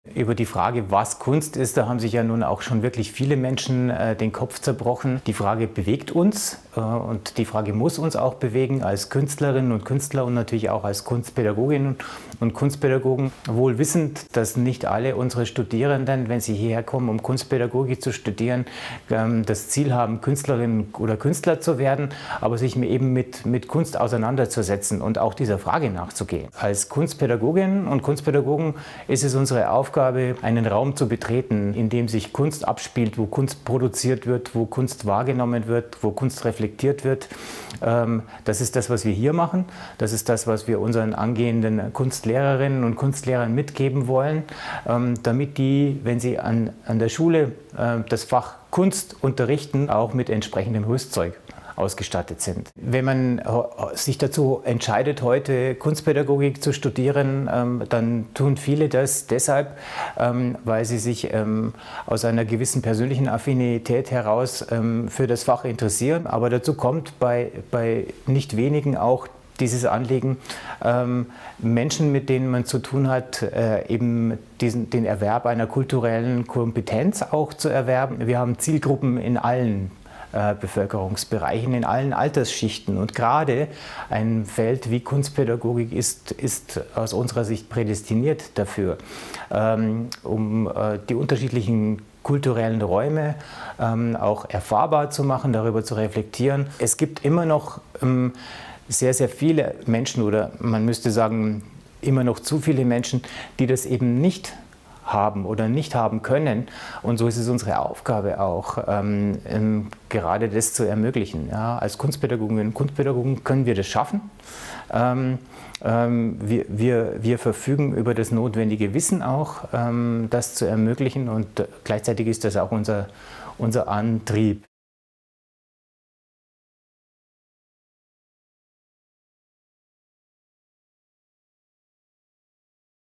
Kleine-Fächer-Woche an der KU: Kunstpädagogik und Kunstdidaktik
Die Aktionswoche „Andere Zeiten – andere Räume“ der KU bietet vom 20. bis 26. Januar 2020 Einblick in die Arbeit und die Relevanz der sogenannten Kleinen Fächer. Dabei handelt es sich um Fächer, die in Deutschland pro Universitätsstandort mit nicht mehr als drei Professuren vertreten sind. Mit der „Kleinen- Fächer-Woche“ will die KU unterstreichen, welche Bedeutung auch diese Themenfelder haben, um aus anderen Zeiten und Räumen neue
Erkenntnisse für die Gesellschaft der Gegenwart zu gewinnen.